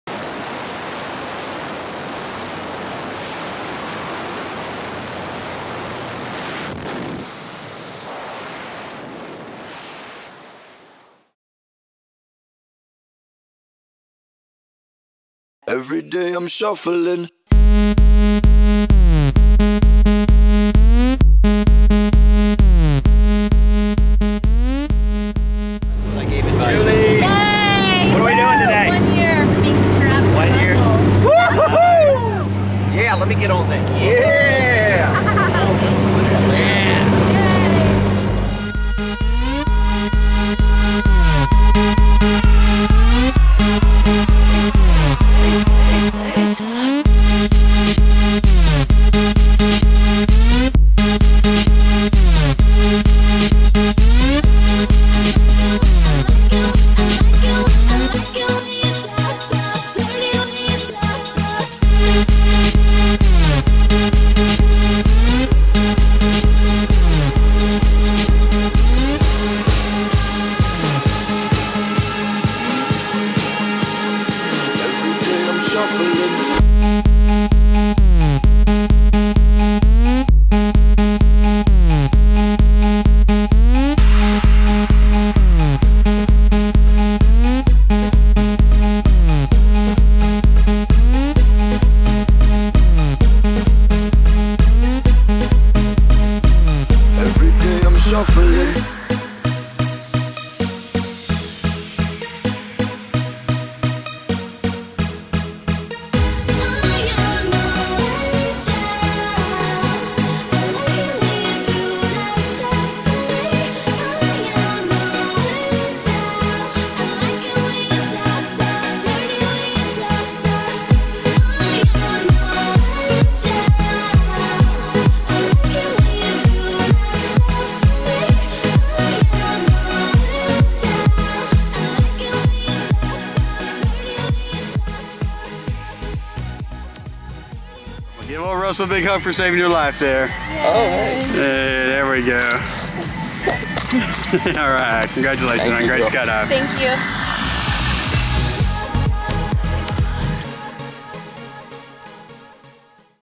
1 year anniversary of skydiving
Thanks to everyone who participated in my 1 year anniversary jump, especially Russell! Sorry for almost flipping into you!